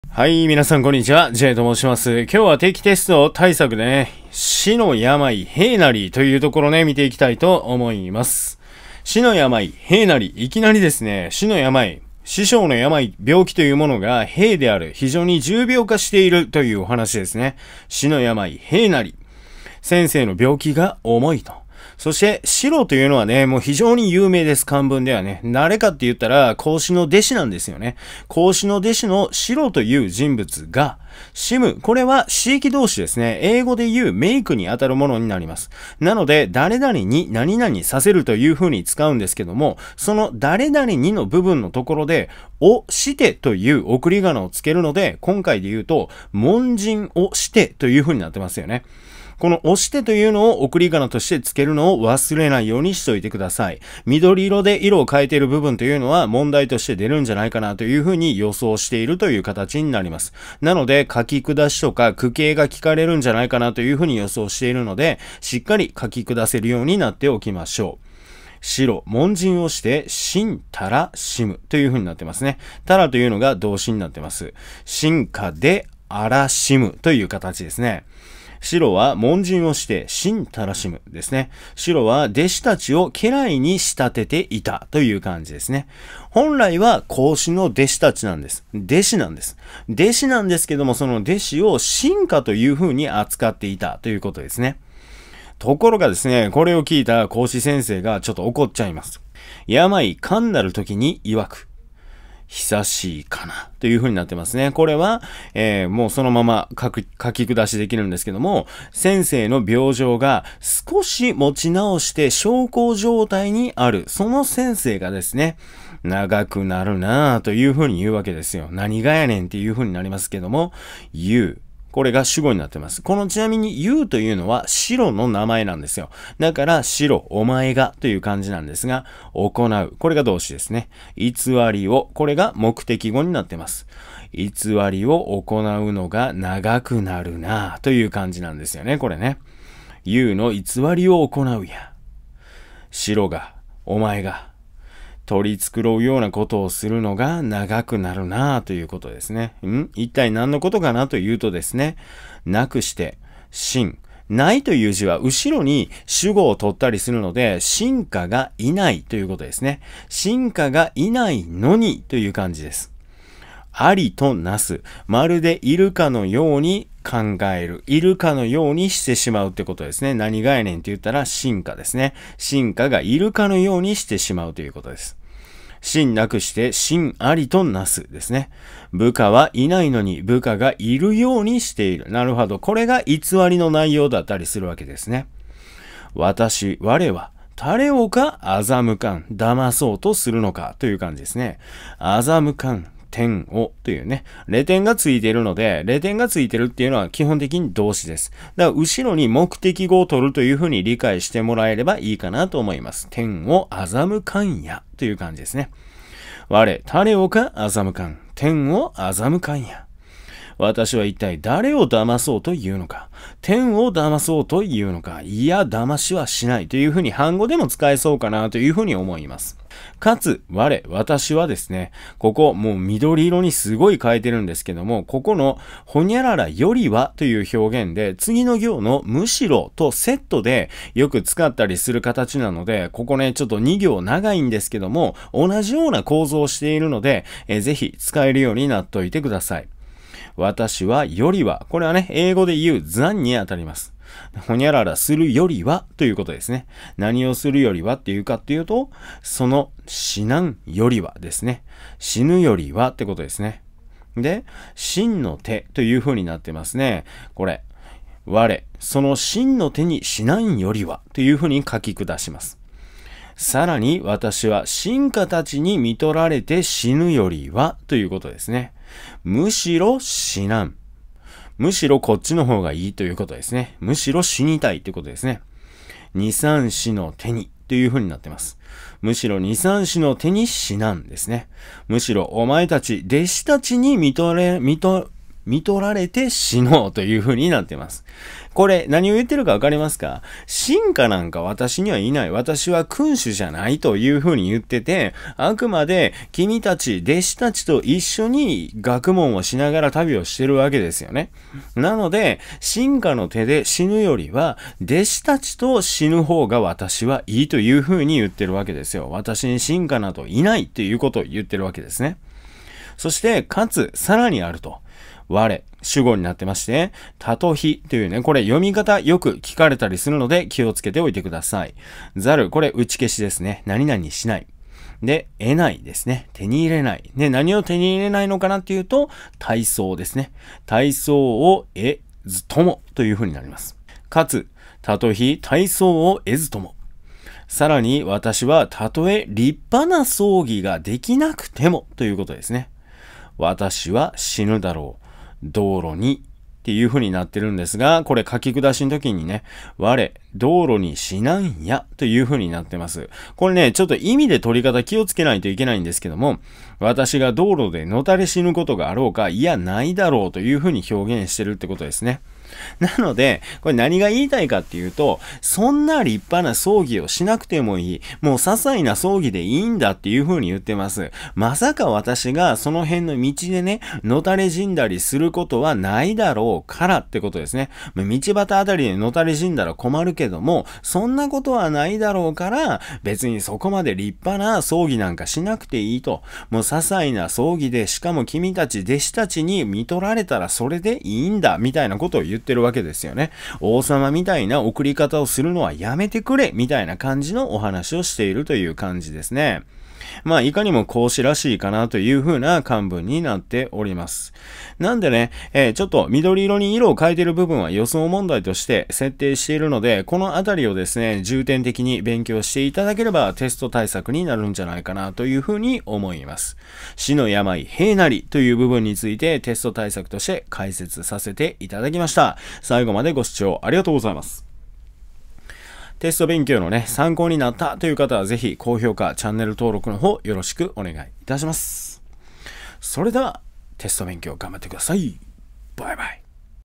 子疾病のわかりやすい現代語訳と書き下し文と予想問題 JTV定期テスト対策
孔子先生は、弟子たちがお好きじゃ＾＾

予想問題などを掲載しています。

使用機材
マイク
audio-technica AT2020＋USB

カメラ
Canon EOS 80D

パソコン
アップル　MacBook Pro 15インチ